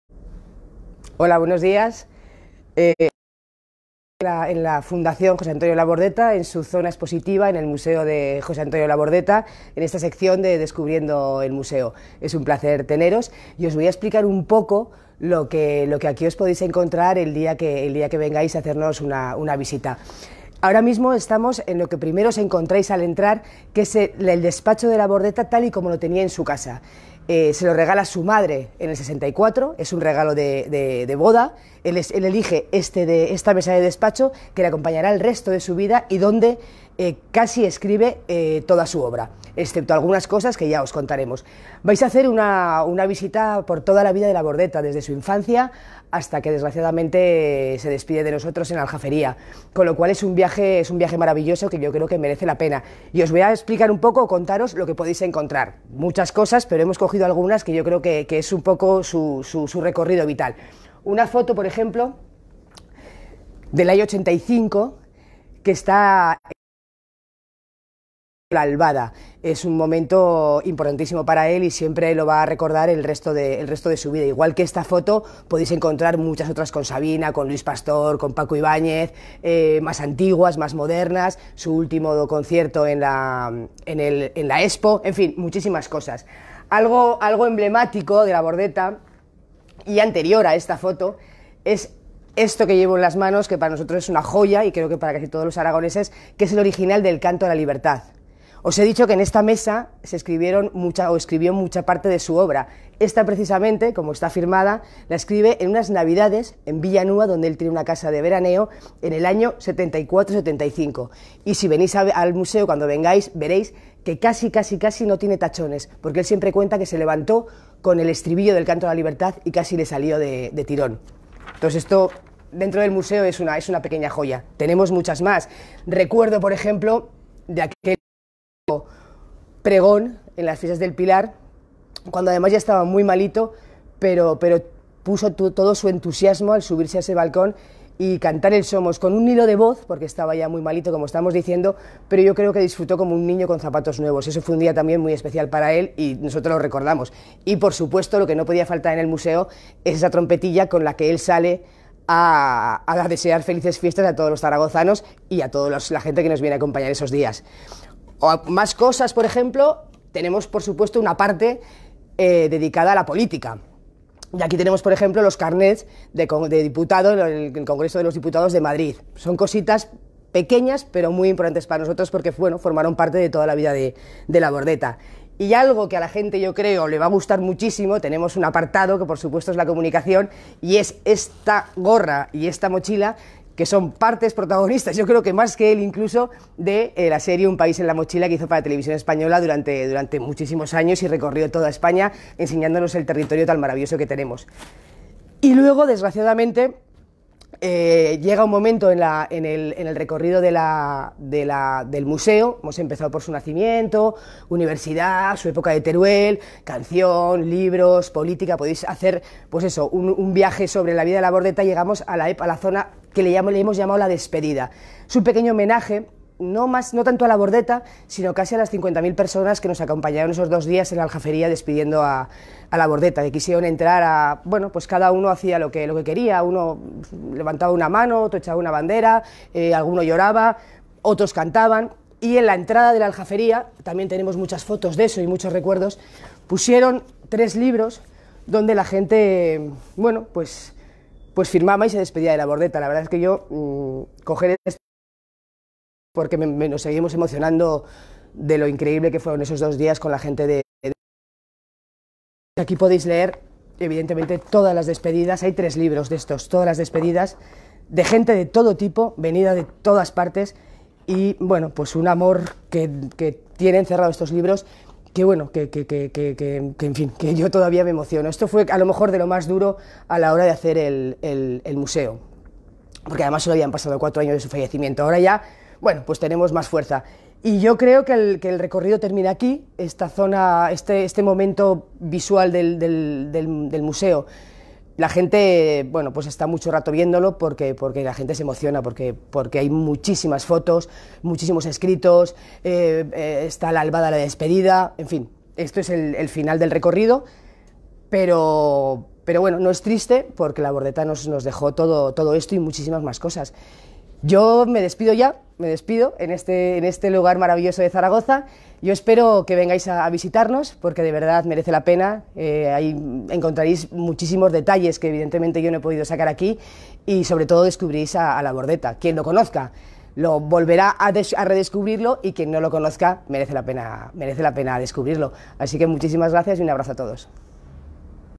DescubriendoelMuseo Museo Fundación José Antonio Labordeta
Vídeo en directo desde el Museo Fundación José Antonio Labordeta con la directora de la fundación, Paula Labordeta. 6 febrero 2020